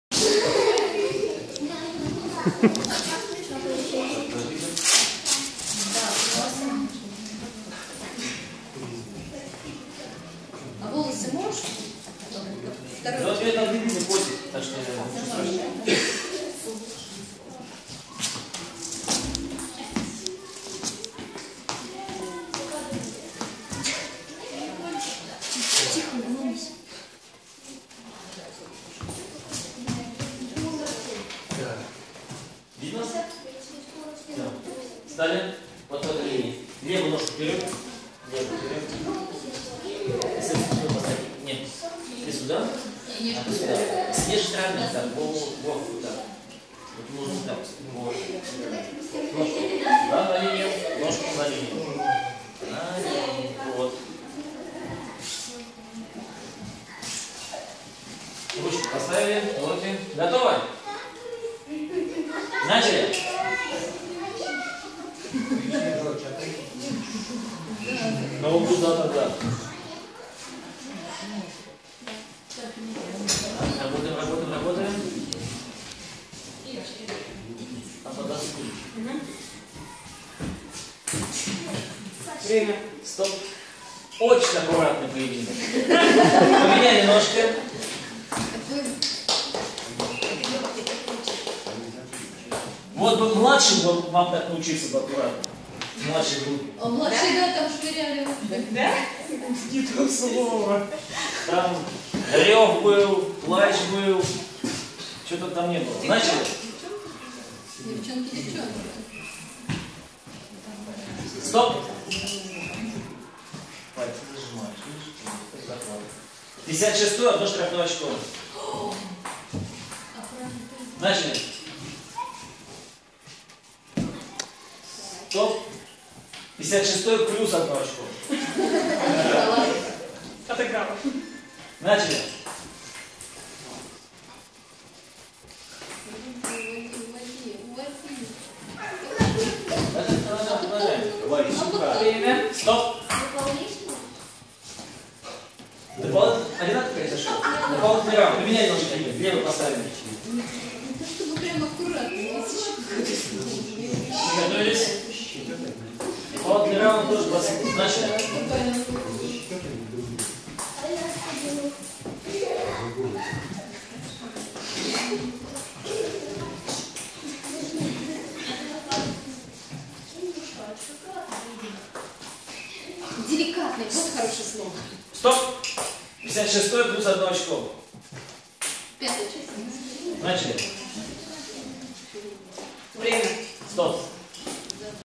Tuishou (Pushing Hands) Competition, 10 years old girls // Туйшоу дети 10 лет
Quite a careful pair! :) // Соревнования Содружества Духовных Воинских Традиций на Соколинке 2015.